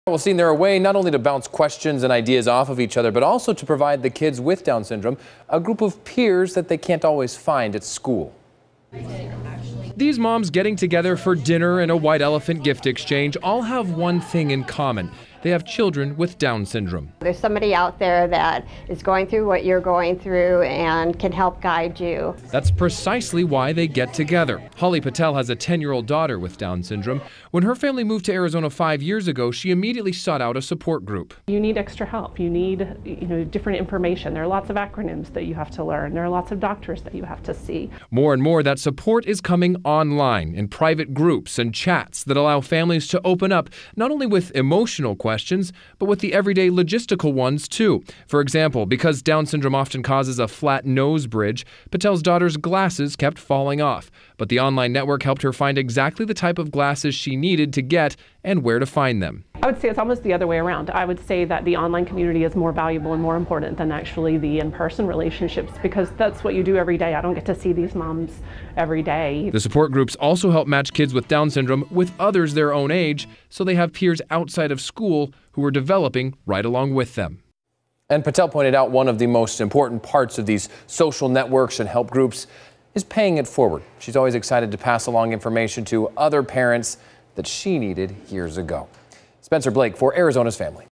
VIDEO: Down syndrome support groups go social
When a child gets a diagnosis as serious and as challenging as Down syndrome, their parents often yearn for support that only other families in a similar situation can give. That’s the whole premise behind Down syndrome support groups. Their presence online has been ballooning in 2018.